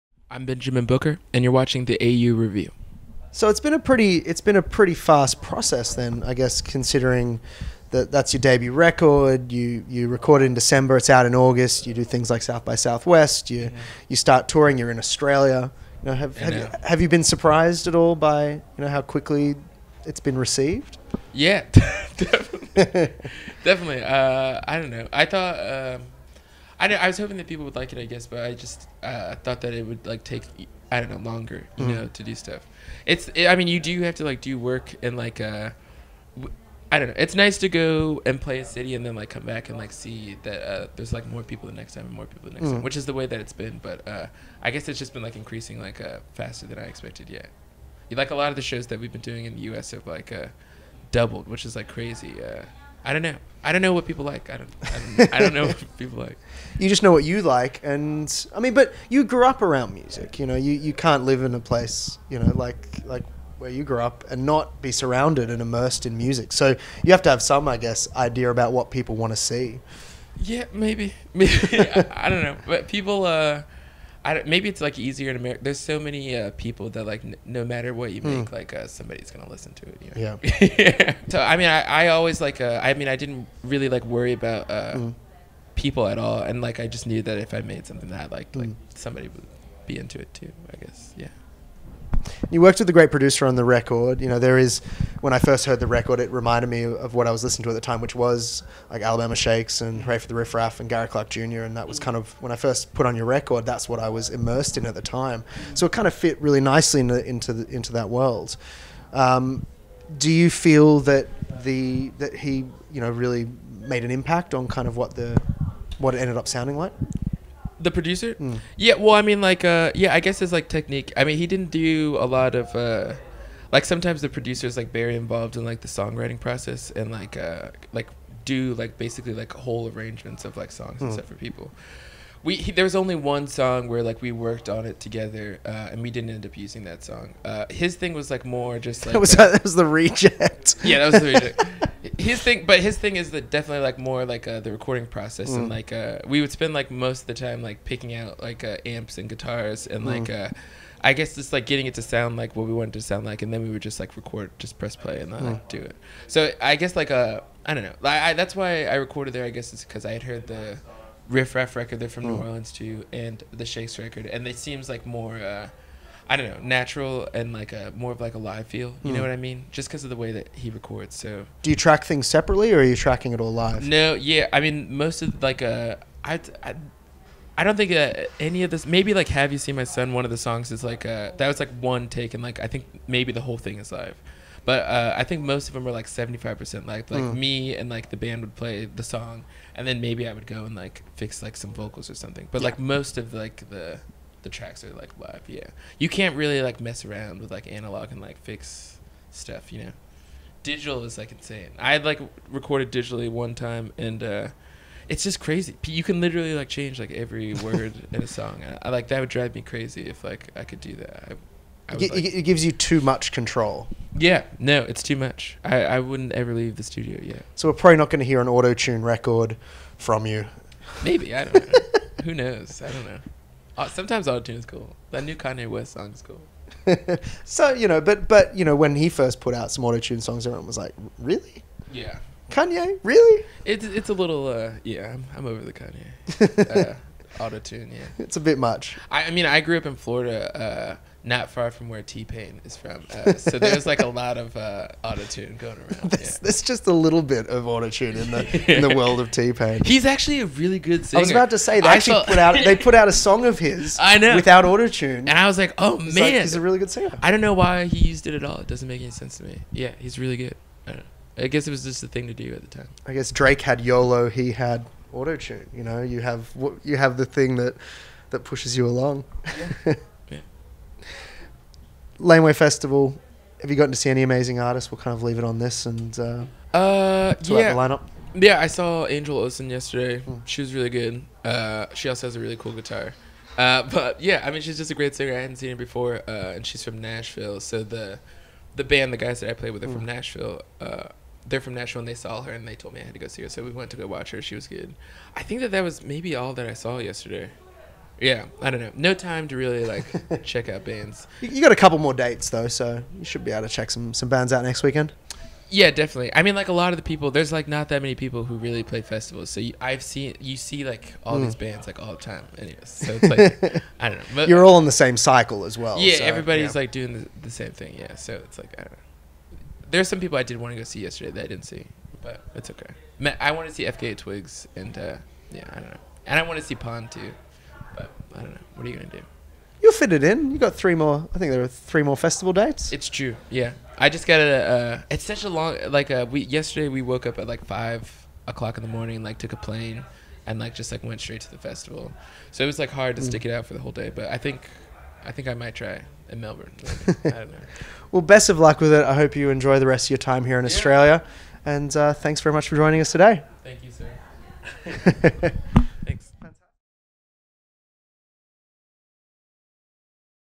Benjamin Booker: Laneway Interview (Part Two)
the AU review sits down with New Orleans artist Benjamin Booker while in Australia as part of the 2015 Laneway Festival lineup.

Interview/Edit: Larry Heath
Camera: Johnny Au

Filmed February 2nd in Sydney, Australia at AU HQ.